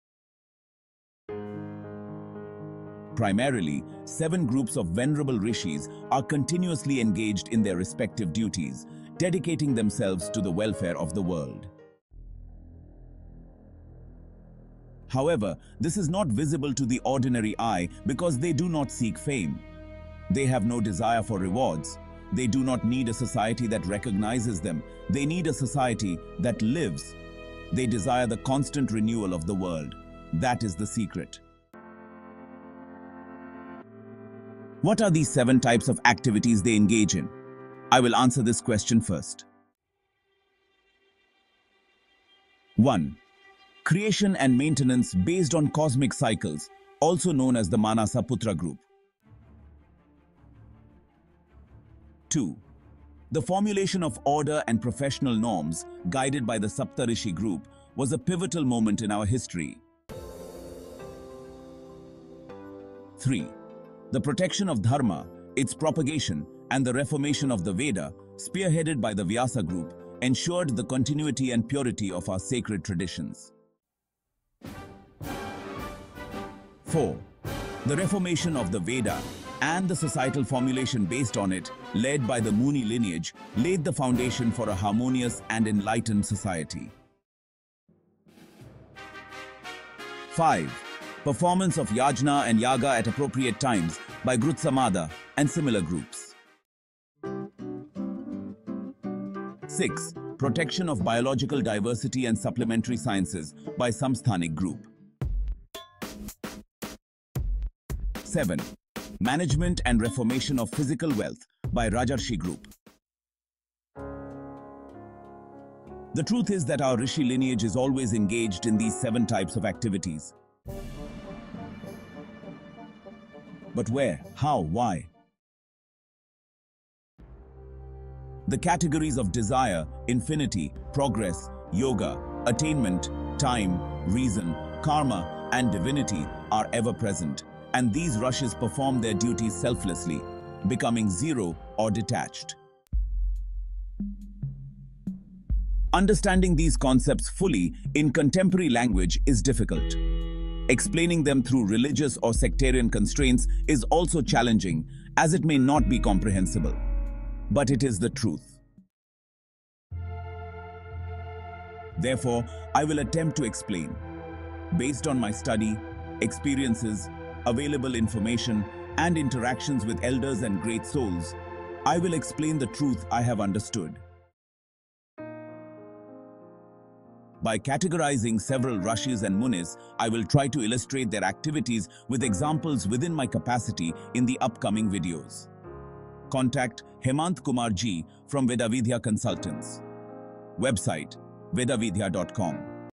The Seven Groups of Rishis
Explore the seven types of Rishis and their eternal duties in this enlightening video. Learn about the different groups and their unique roles in maintaining cosmic balance and societal well-being. Discover the timeless wisdom of these revered sages and their impact on the world.

Hemanth Kumar G
Vedavidhya Consultants & Brahmi Ayurveda Clinic, Chikmagalur